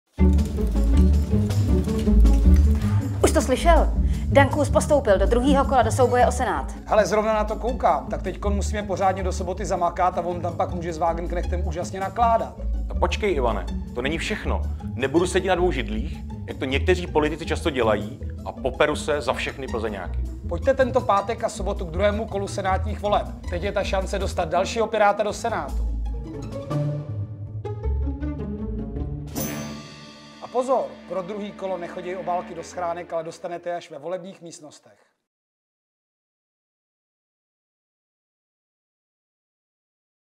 Volte Daniela Kůse do Senátu!
Díky Plzeňákům se podařilo dostat do boje o Senát i Daniela Kůse! Prosíme, pomozte nám jej o víkendu zvolit, ať posílíme Piráty v Senátu a oni tam můžou úžasně nakládat. :)